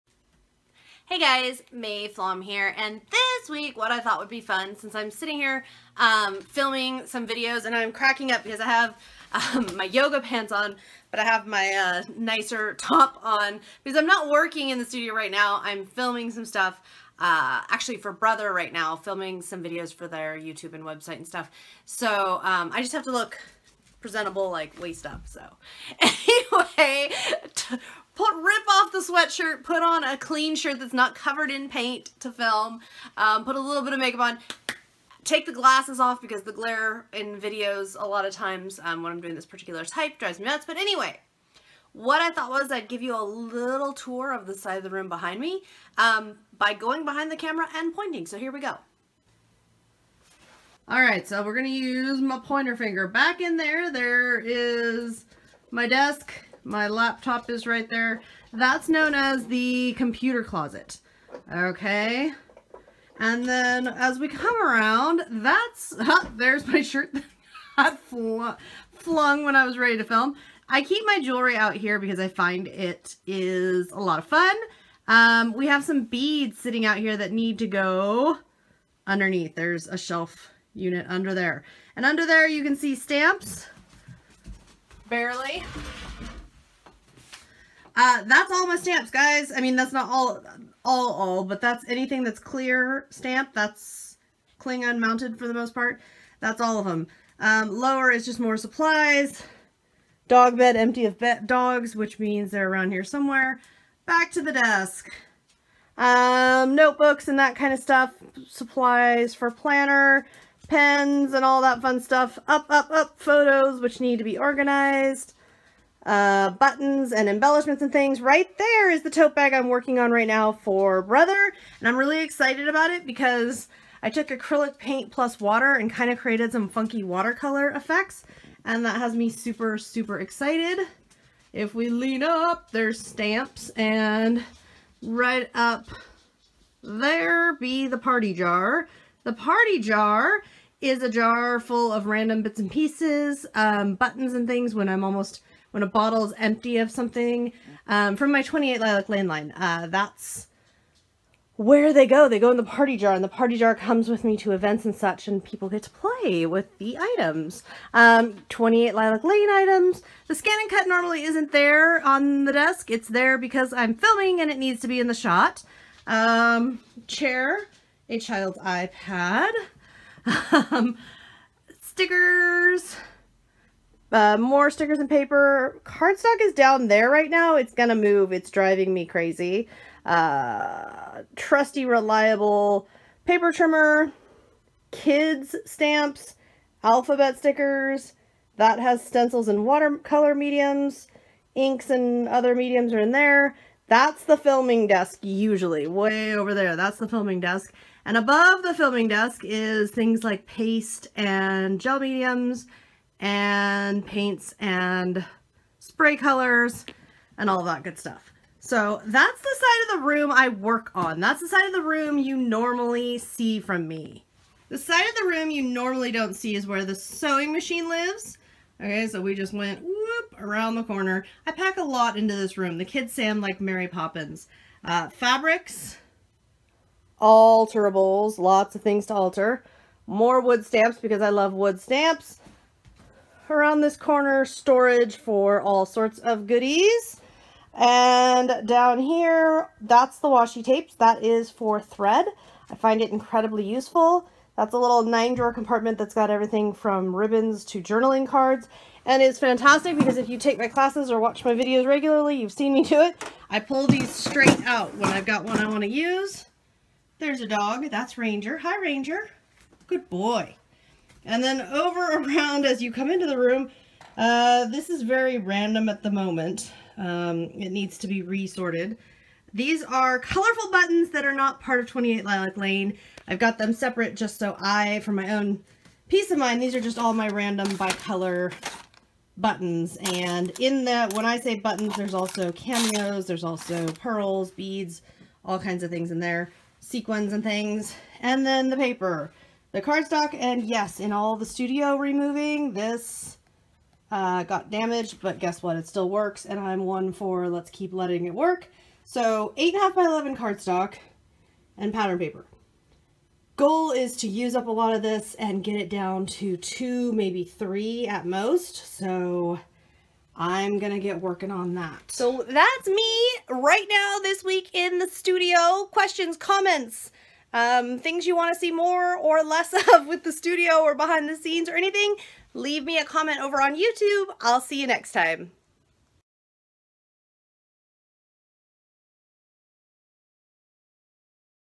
In the studio: February 1
Hello everyone! This week I'm showing you around the studio, as it is cleaner than usual. Have a great week!